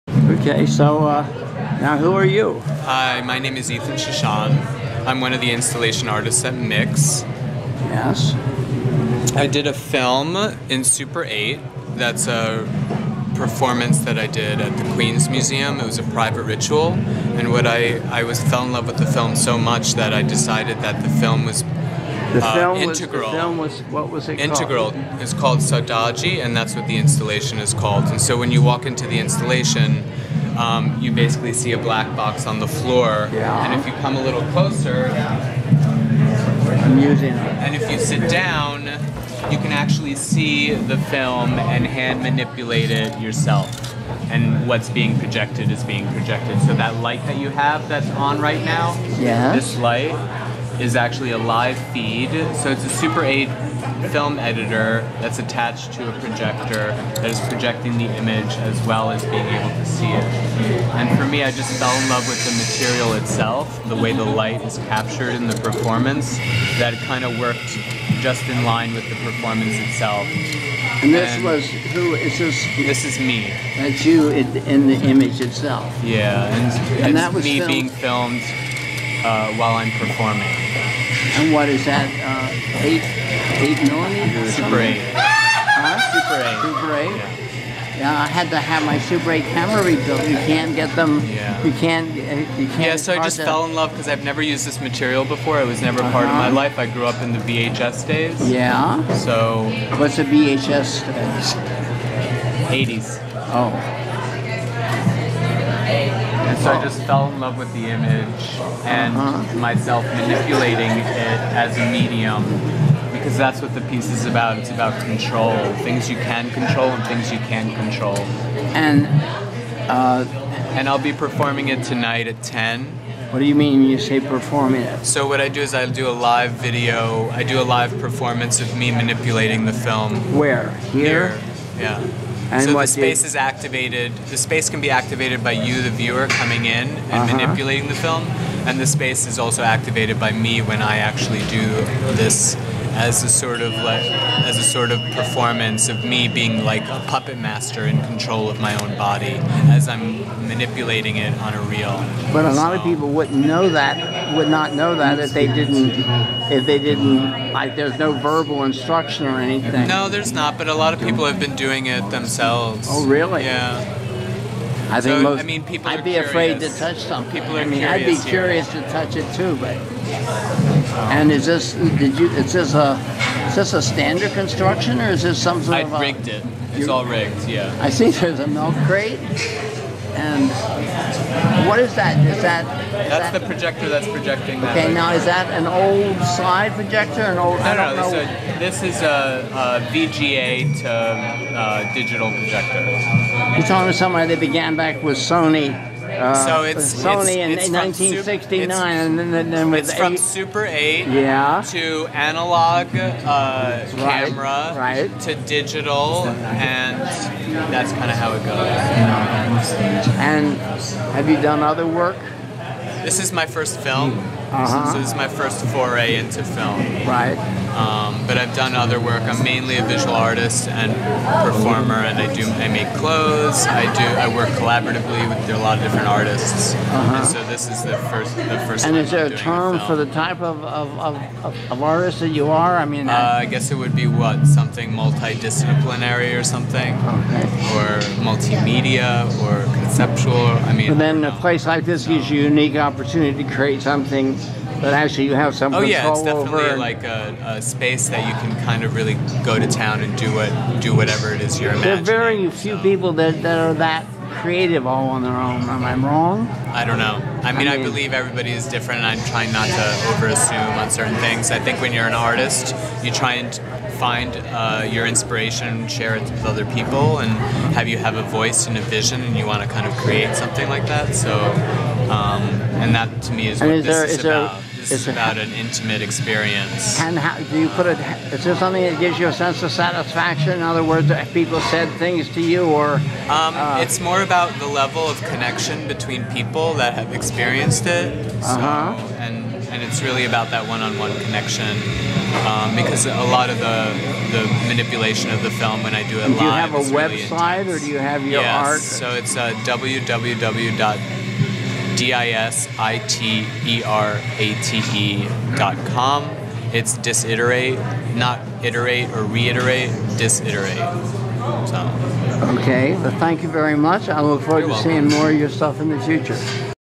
SAUDADE by ETHAN SHOSHAN
Ethan Shoshan explains his multi-media "installation" at NYC's Mix Queer Experimental film festival.  His artistic work can be found at disiterate (dot) com.